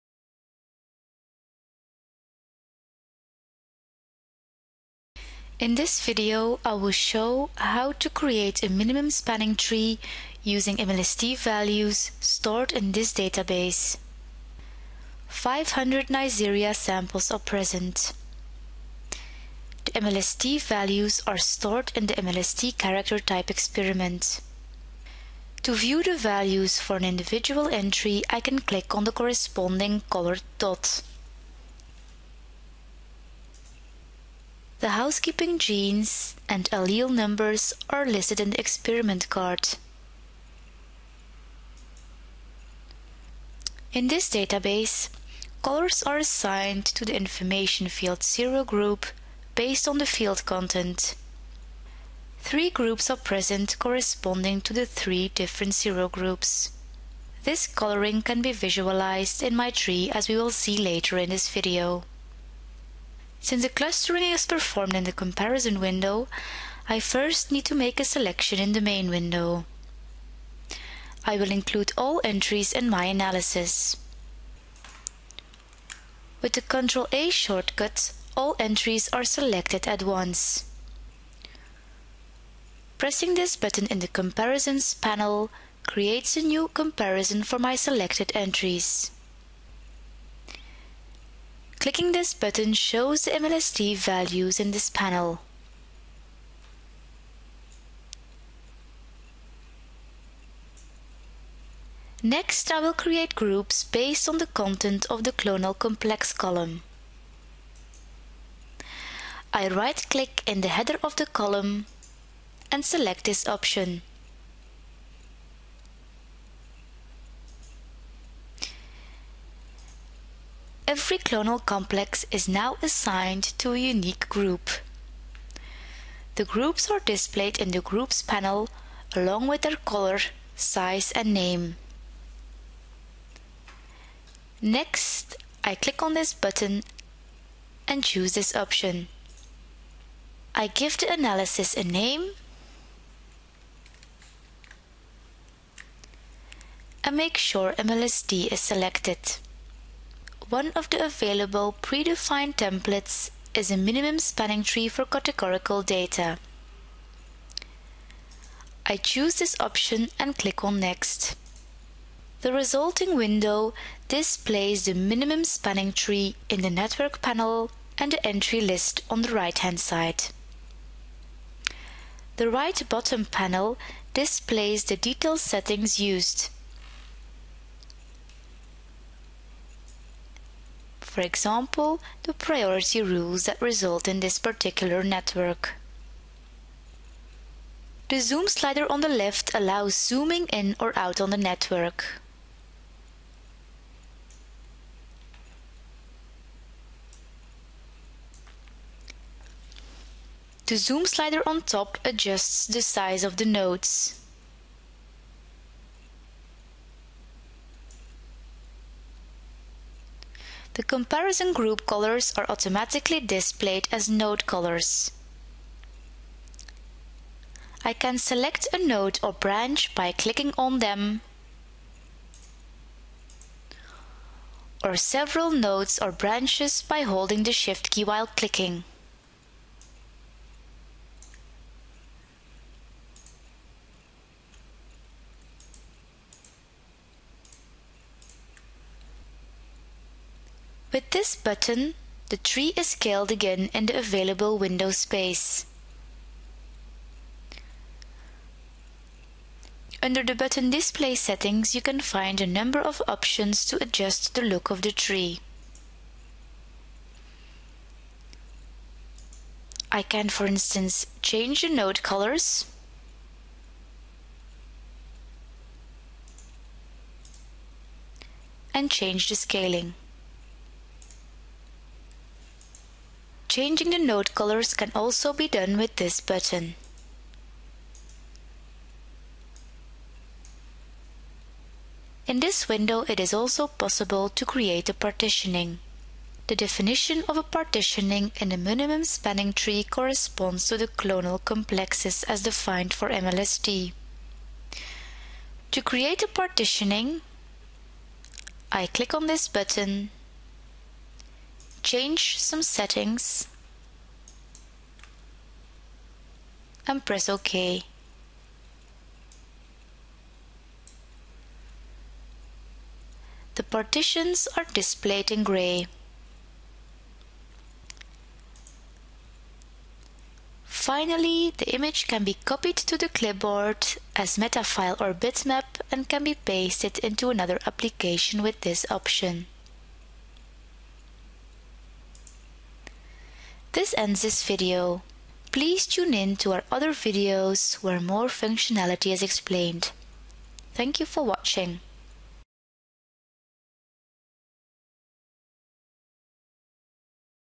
Create a minimum spanning tree based on MLST data (BioNumerics 7)
This video shows how to create a minimum spanning tree based on MLST allele numbers. The same steps are also applicable for clustering of other character data sets such as MLVA/VNTR.